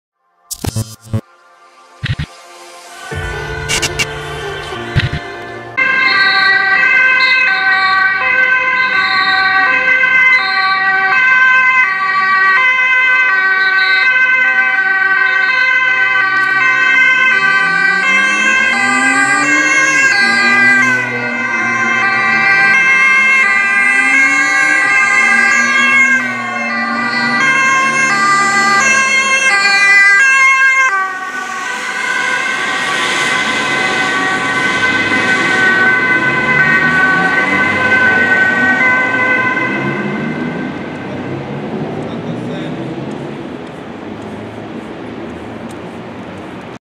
20-1-2018 || Spoedtransport vanuit incident in Gouda naar het Erasmus Medisch Centrum Rotterdam.
› In deze video ziet u een spoedtransport vanaf een ernstig incident in Gouda naar het Erasmus Medisch Centrum in Rotterdam.

Gefilmd door: Nick